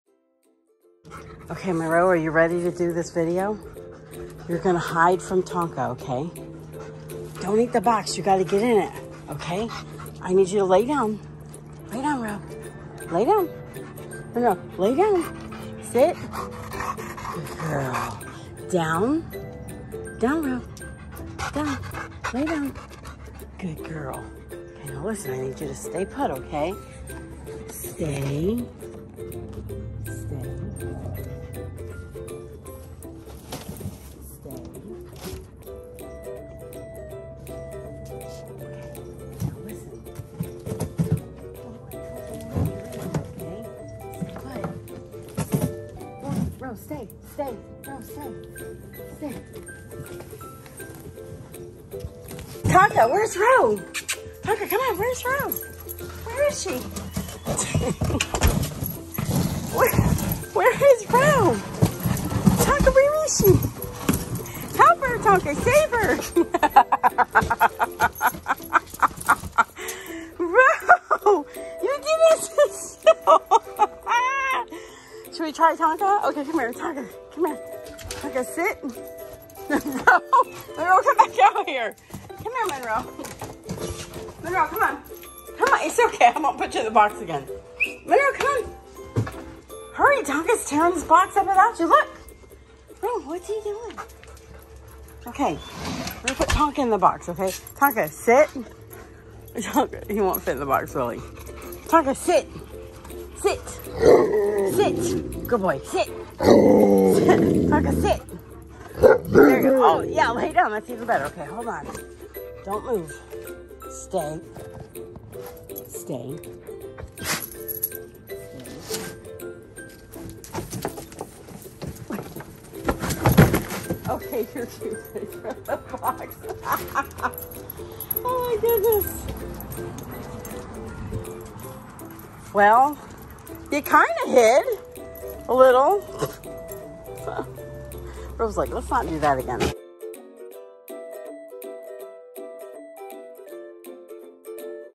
Husky Reluctantly Hides From Malamute
I thought this would be a good idea but Monroe did Not agree with me 🤣🤣🤣🤣❣️